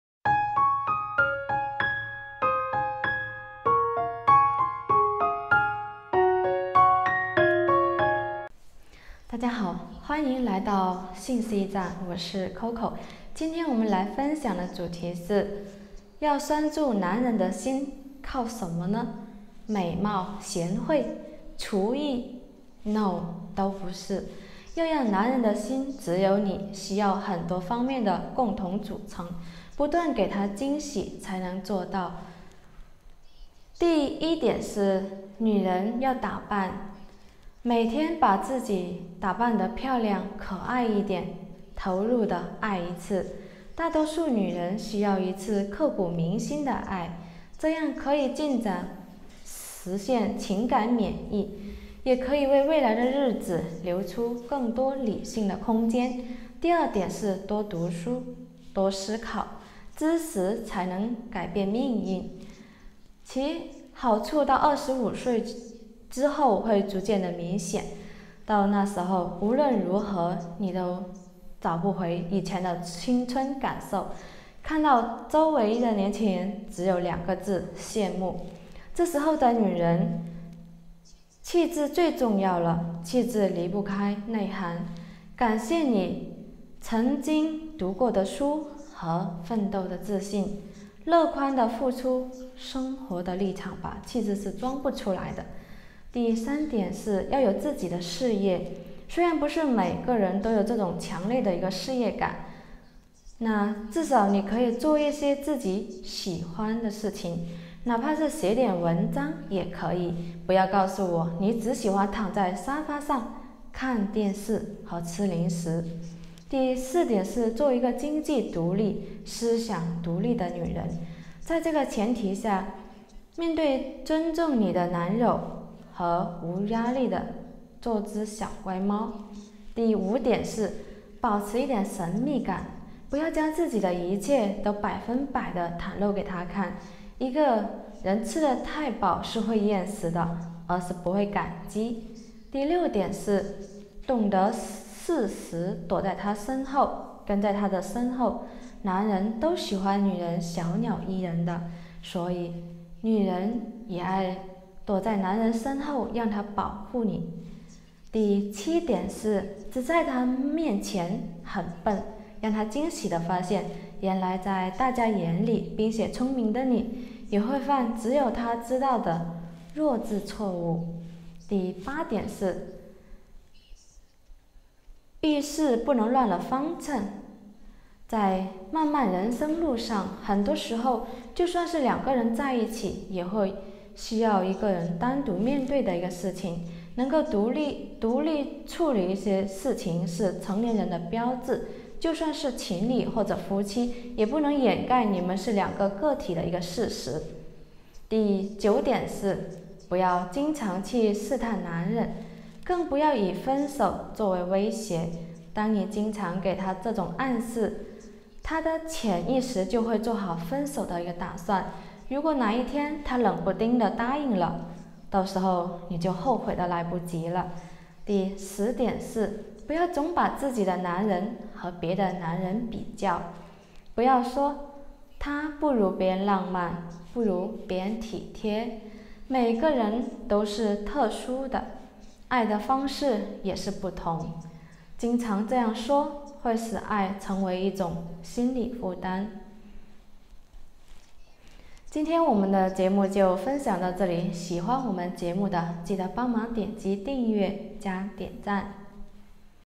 要拴住男人的心靠什麽呢 What do you want to hold on to the man’s heart?）
保持一点神秘感。
不要将自己的一切都百分之百地袒露给他看，
一个人吃得太饱是会厌食的，而不会感激